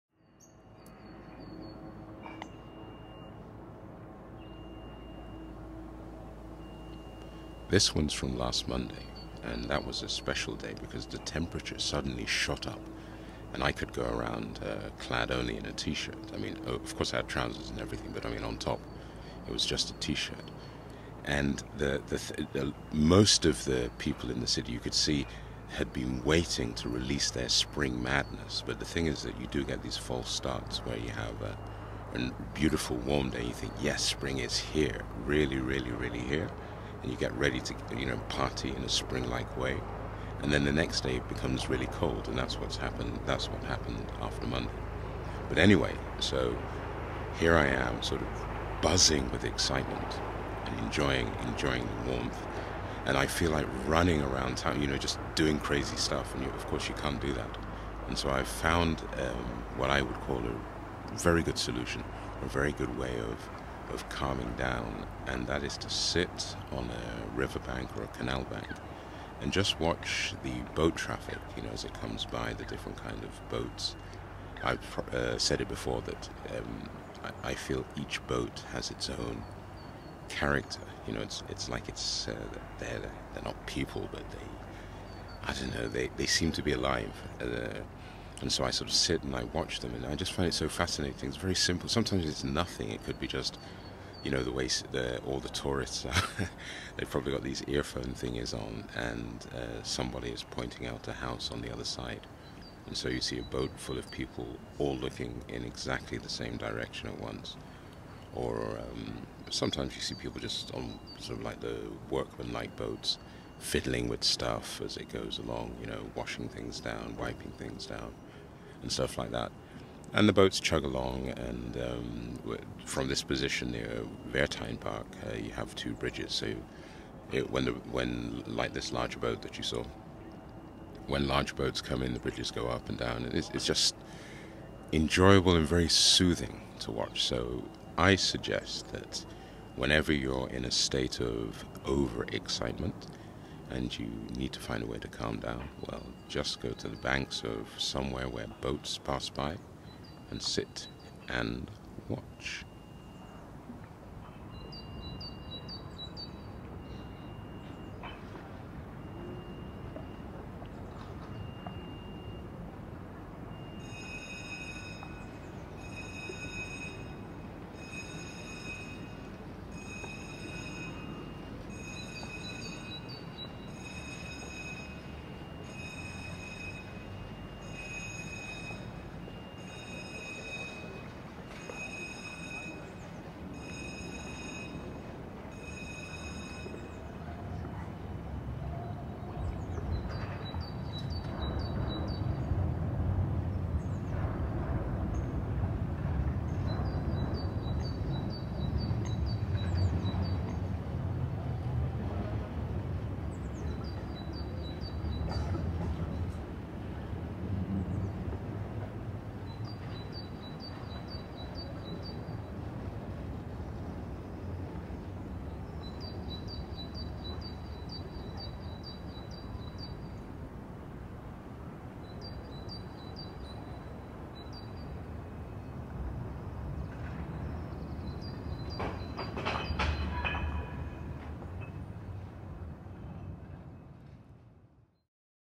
Amsterdamica S02E50: Zzzing
The first day out wearing nothing but a t-shirt. I’m trying my best to stay calm and not zoom around the city like Roadrunner: that’s what springtime can make you do!
Anyway...
One of my favourite cooling down techniques is to sit on the bank of a canal and watch the boats go by.